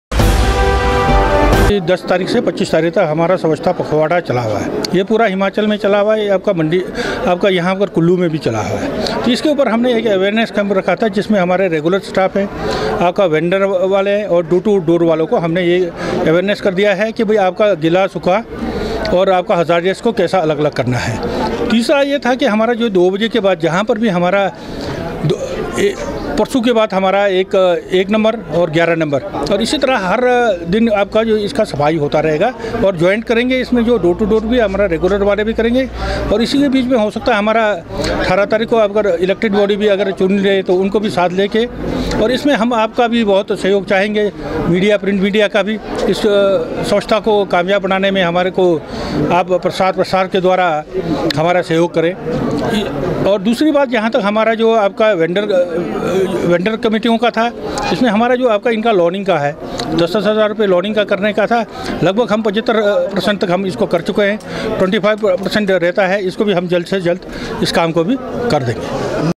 कुल्लू में रेहड़ी फहड़ी वालो को बताया कानून
नगर परिषद कुल्लू के द्वारा सरवरी में आयोजित किया कार्यक्रम